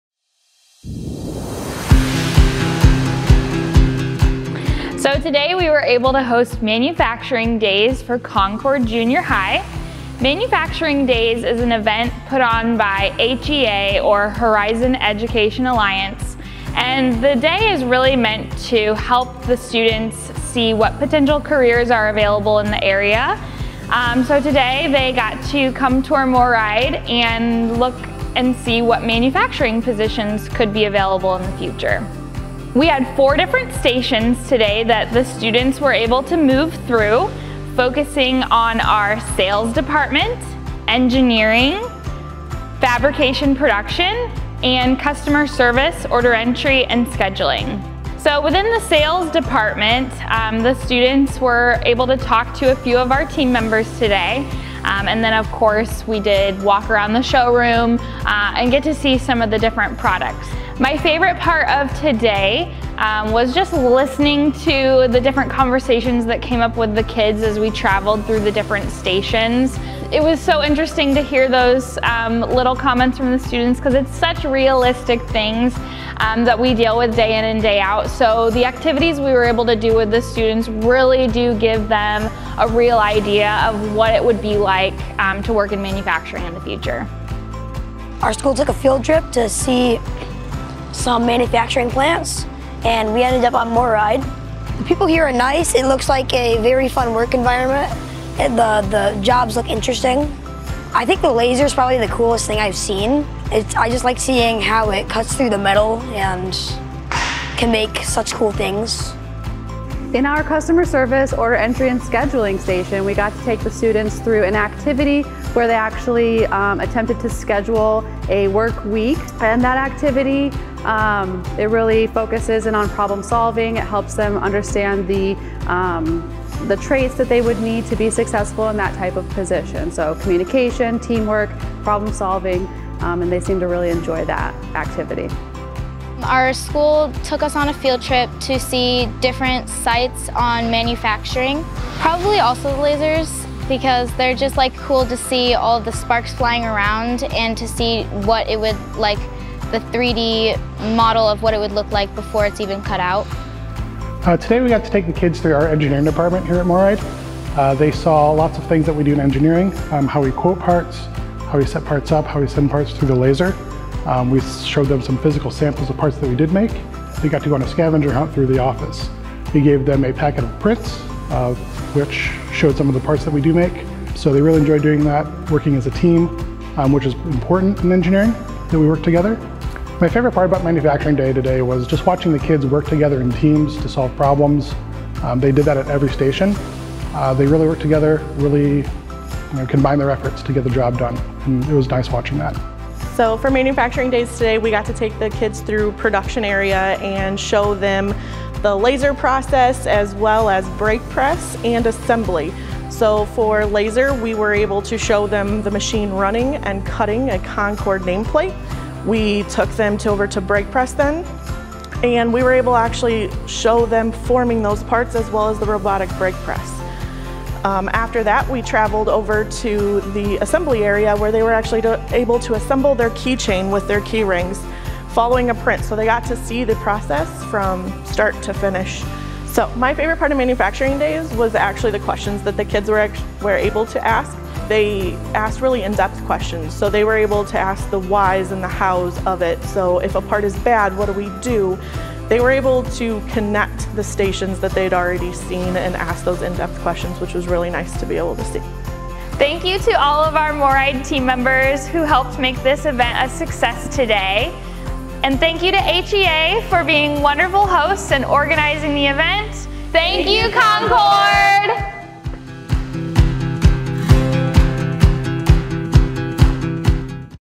Manufacturing Day 2022
“It’s another Manufacturing Day at MORryde!

The goal for MFG Day is to inspire and introduce local students to the various manufacturing-related career opportunities in their area. This year, we were visited by Concord Junior High School where they had a chance to tour our MORryde campus and learn a bit about our processes.

“It looks like a very fun work environment and the jobs are interesting” – Jude, Concord Junior High School.